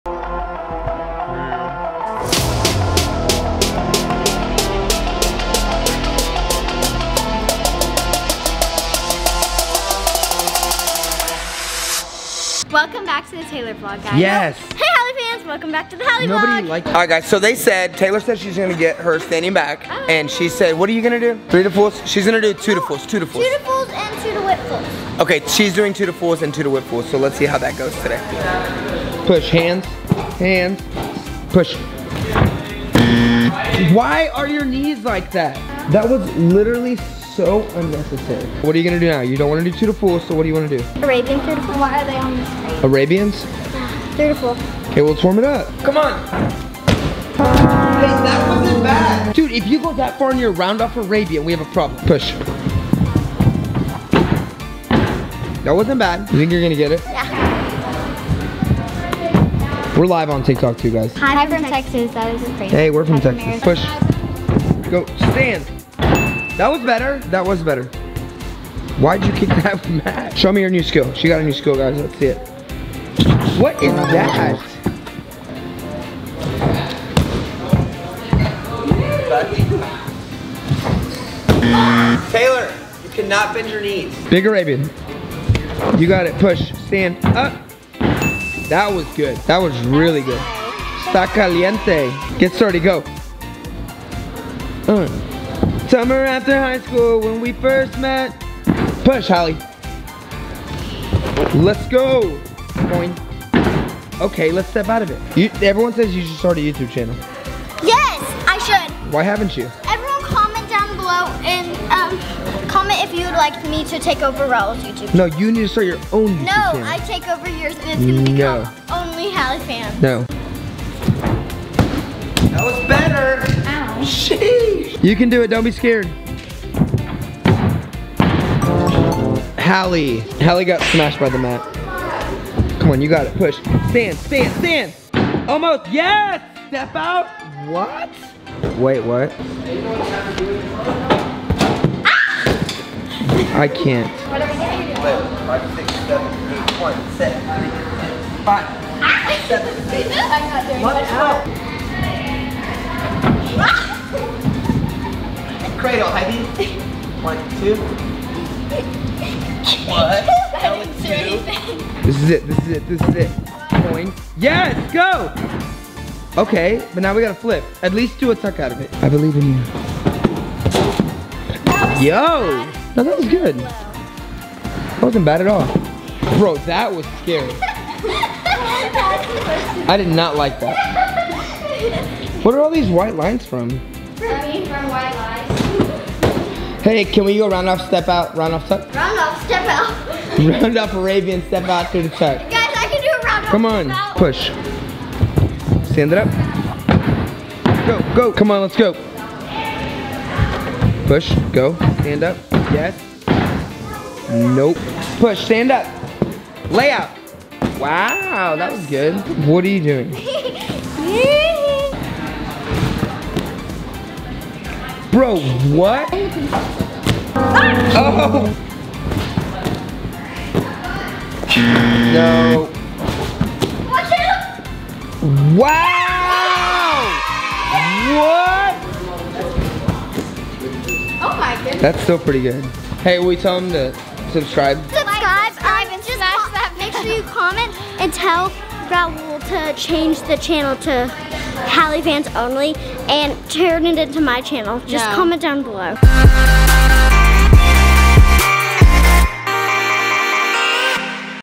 THIS IS THE MOST RANDOM TUMBLING SESSION EVER!
Hallie and Taylor in the gym working skills of all kinds lol!